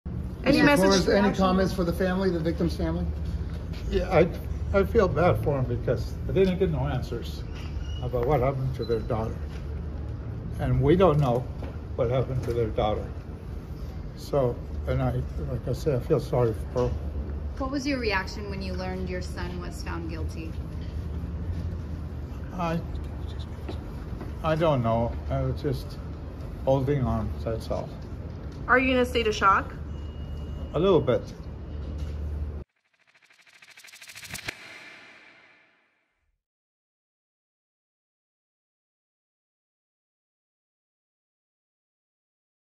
Short | Ruben Flores has message after 'not guilty' verdict in Kristin Smart trial
On Oct. 18, 2022 Ruben Flores was found not guilty of being an accessory to the crimes of his son, Paul Flores in the murder trial of Kristin Smart.